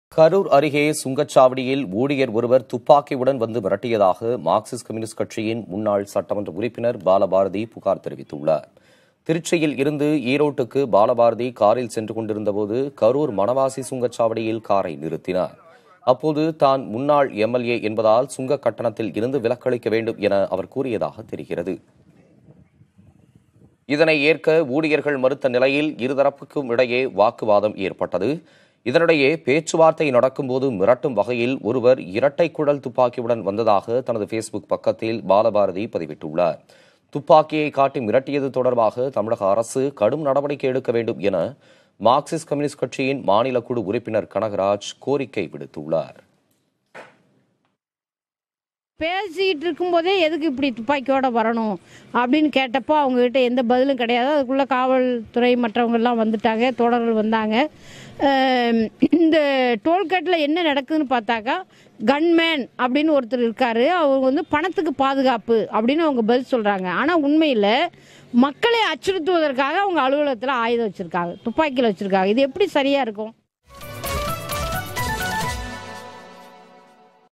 கொள்ளையடிப்பதை நிறுத்துங்கள்... ஒத்தையடி ரோட்டுக்கு சுங்க கட்டணமா? | தோழர் கே.பாலபாரதி | CPIM
கொள்ளையடிப்பதை நிறுத்துங்கள்... ஒத்தையடி ரோட்டுக்கு சுங்க கட்டணமா?

மணவாசி டோல்கேட்டில் முன்னாள் சட்டமன்ற உறுப்பினர்
தோழர் கே.பாலபாரதி வாகனத்தை நிறுத்தி, பேசிக்கொண்டிருந்தபோது திடீரென ஒரு நபர் இரட்டை குழல் துப்பாக்கியை நீட்டி மிரட்டியுள்ளார்.

துப்பாக்கியை நீட்டும் உரிமையை ஒரு டோல் கேட் நிர்வாகத்திற்கு கொடுத்தது யார்? ஊர் அறிந்த தலைவருக்கே இதுதான் நிலைமை என்றால் சாமானியர்களிடம் எத்தனை மோசமாக நடப்பார்கள்? இது குறித்து கரூர் மாவட்ட ஆட்சியர் உடனடியாக தலையிடுவது அவசியம். துப்பாக்கியை நீட்டி மிரட்டுவது எந்த வரைமுறையிலும் வராது. அராஜகம், கொலை முயற்சி தவிற வேறல்ல.

தமிழக முதலமைச்சர் உடனடியாக இப்பிரச்சனையில் தலையிட்டு துப்பாக்கி வைத்திருக்கும் அதிகாரத்தை யார் கொடுத்தது என விசாரிக்க உத்தரவிட வேண்டும். ஒருவேளை உரிமம் பெற்றிருந்தாலும் கூட, இதுபோன்ற இடங்களில் பயன்படுத்துவதற்கான அனுமதியை சட்டம் வழங்குவதில்லை. துப்பாக்கிகளை பறிமுதல் செய்து, உரிமம் ரத்துச் செய்ய உத்தரவிட வேண்டும் என வலியுறுத்துகிறோம்.

நடைபெற்றுள்ள நிகழ்வு அனுமதிக்க முடியாத ஒன்று. அதிர்ச்சி தருவது. வன்மையான கண்டனங்கள்.